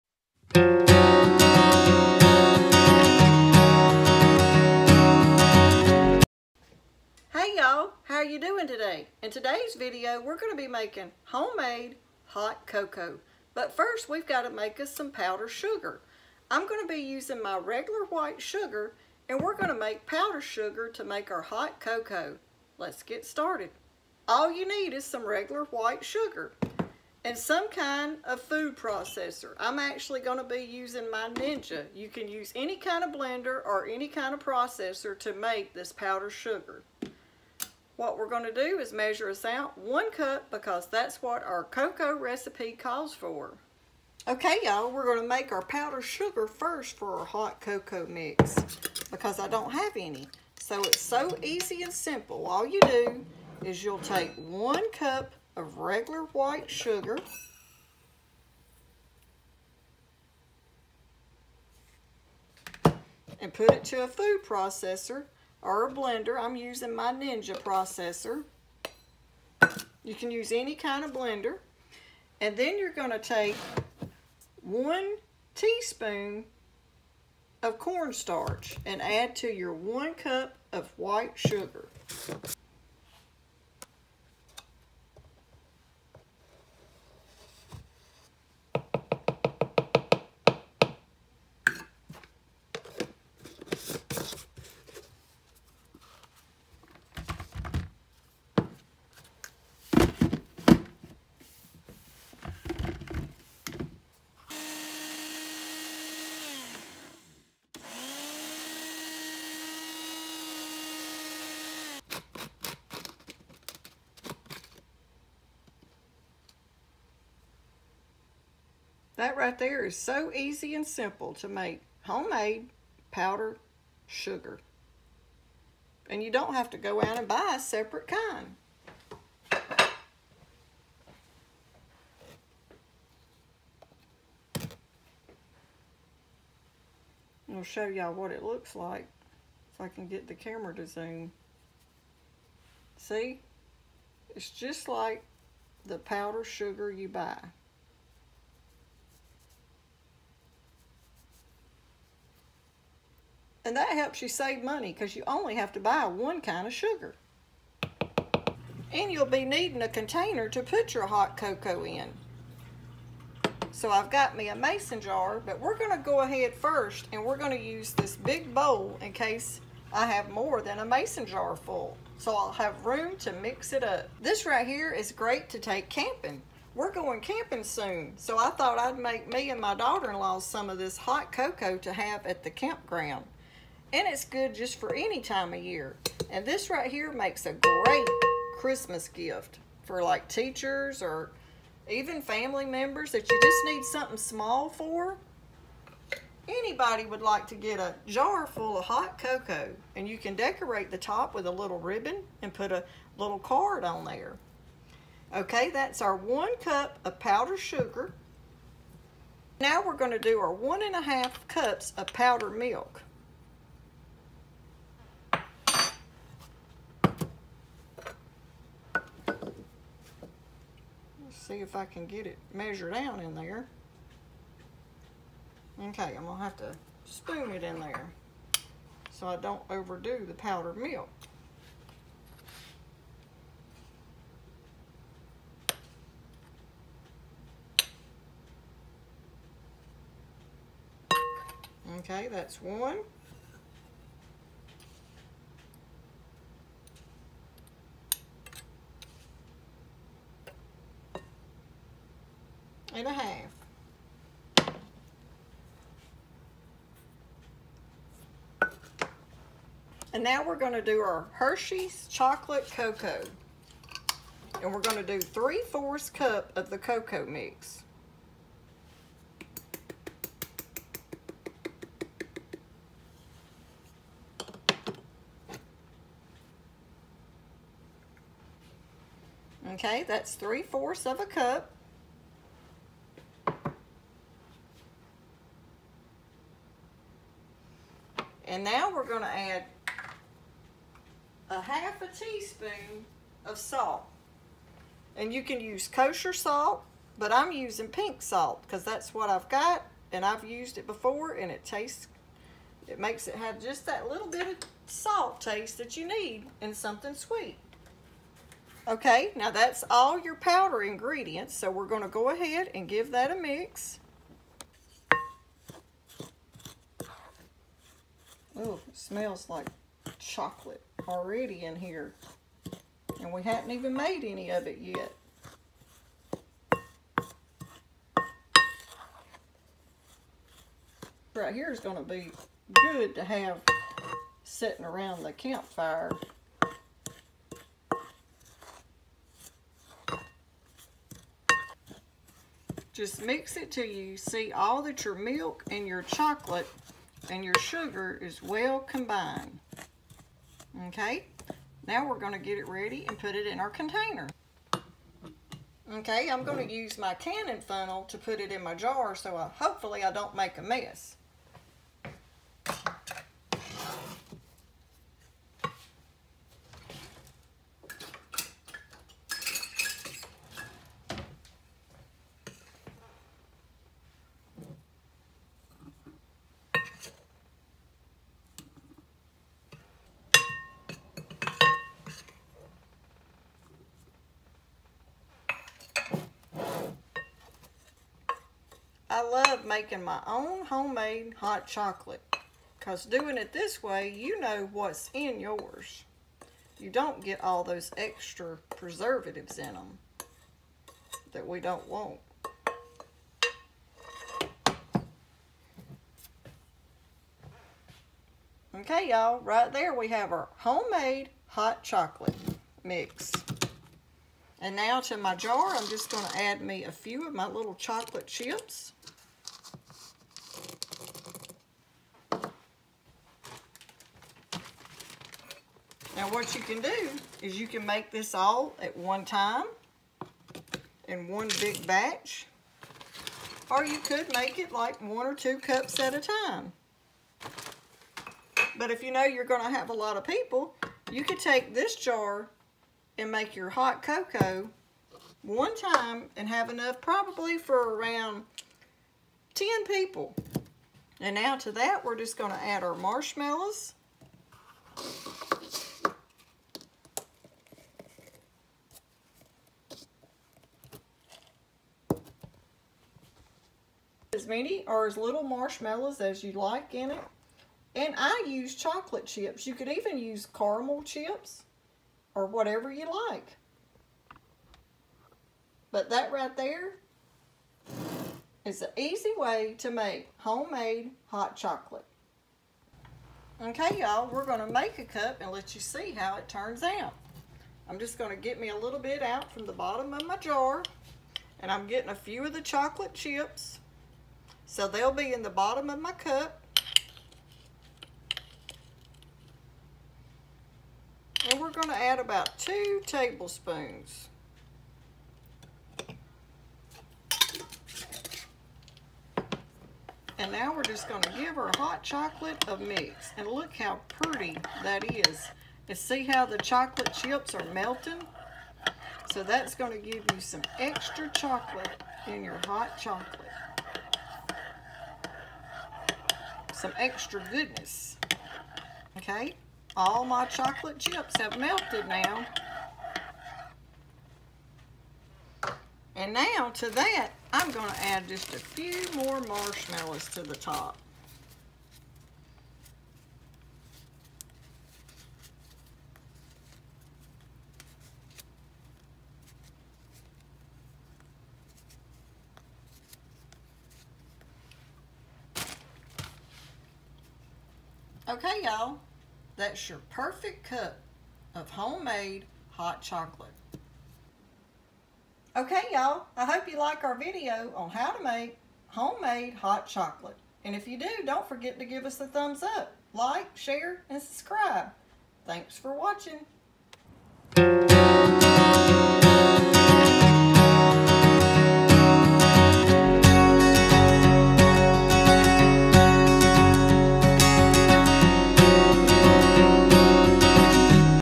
Homemade Hot Cocoa Mix In A Jar-Christmas Gift and Pantry Staple! Easy Quick & Delicious-Save Money
How to make homemade hot cocoa

Ingredients
1 cup of powdered sugar
1 1/2 cups of powdered milk
3/4 cups of Hershey’s cocoa
1 teaspoon of salt
Chocolate chips and marshmallows

Easy quick and delicious

Make it homemade

It’s easy and simple